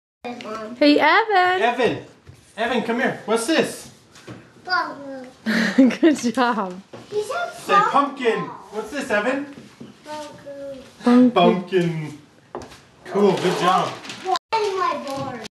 Pumpkin!
Evan says "pumpkin" Halloween 2009- carving pumpkins